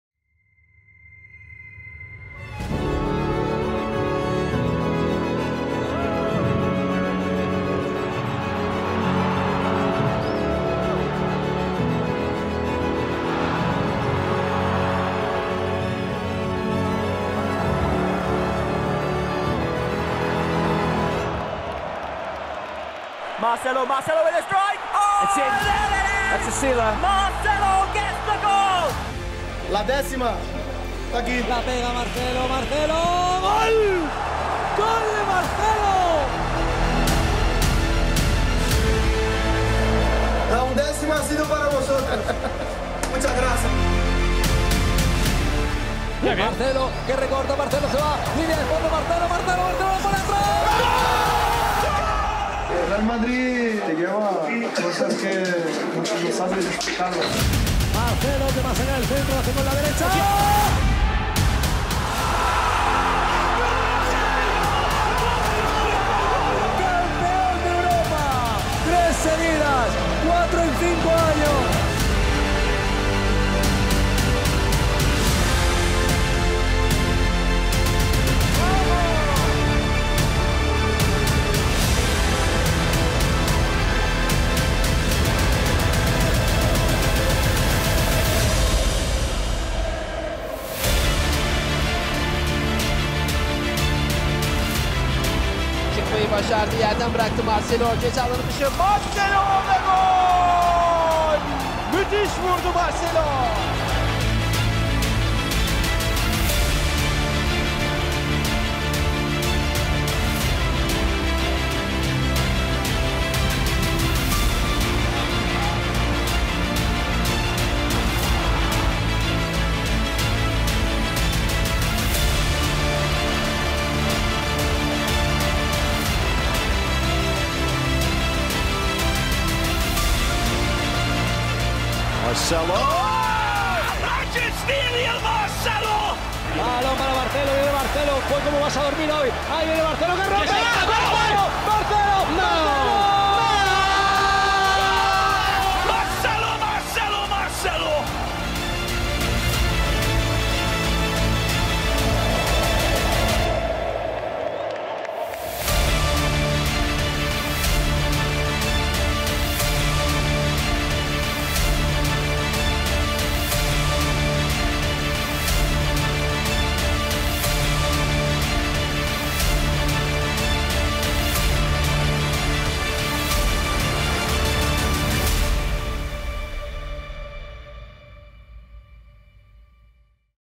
THANK YOU, MARCELO | Real Madrid Legend
Thank you, Marcelo. Real Madrid pays tribute to captain Marcelo on the day of his farewell. Marcelo, who joined Real Madrid from Fluminense in 2006, leaves the club as the most decorated player in Real Madrid history. Truly a legend, having picked up 25 titles. Marcelo’s impressive list of Real Madrid honours includes 5 Champions League titles, 4 Club World Cup titles, 3 UEFA Super Cups, 6 LaLiga titles, 2 Copas del Rey and 5 Spanish Super Cups. Thank you, captain.